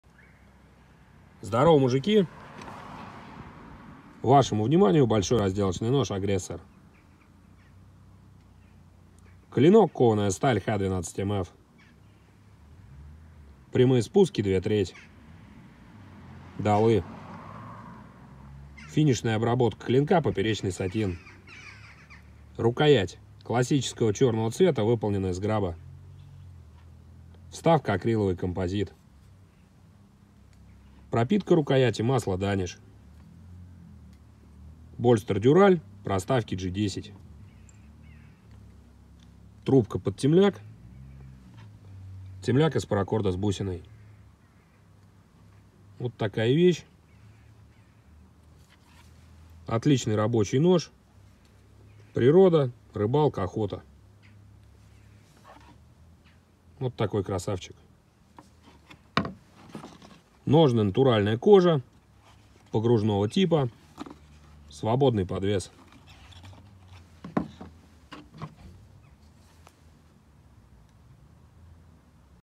Нож «AGRESSOR»
______________________________________ Клинок - кованая сталь х12мф
Рукоять - черный граб/акриловый композит (пропитка Danish oil)
Больстер - дюраль/G10
Темляк - паракорд с бусиной
Ножны - кожа